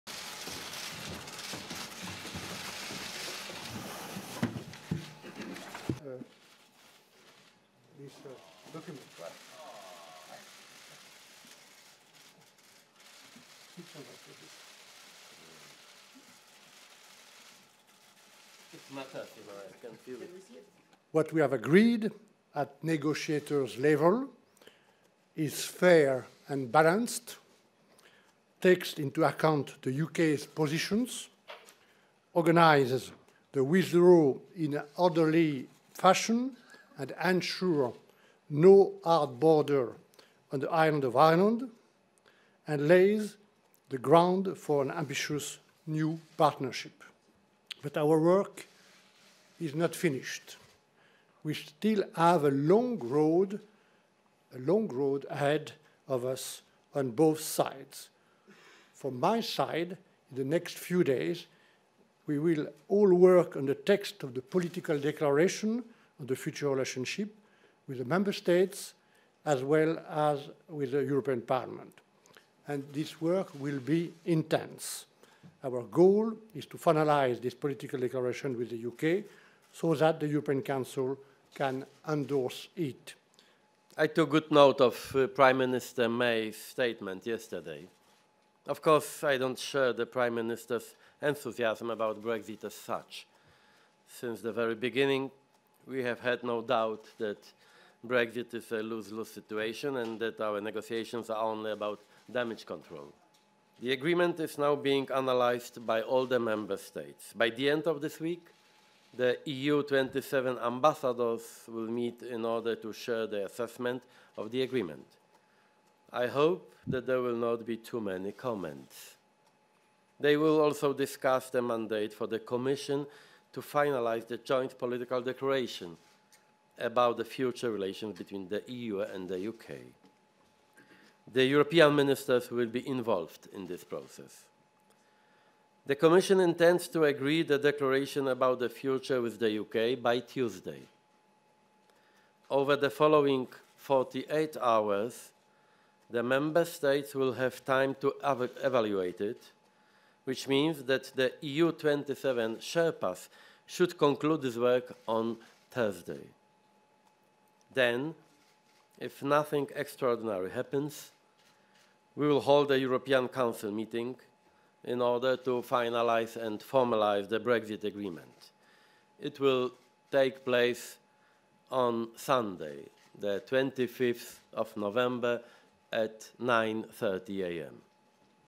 EU summit take place on November 25 to endorse Brexit agreement
European Union leaders will meet on November 25 to endorse the divorce deal with Britain and a political declaration on the future relationship, the chairman of EU Donald Tusk said on November 15.
Tusk said the draft treaty was now analysed by all EU member states and that by the end of this week their ambassadors would meet to share comments and give the European Commission a mandate to finalise the joint political declaration about the future relations between the EU and Britain.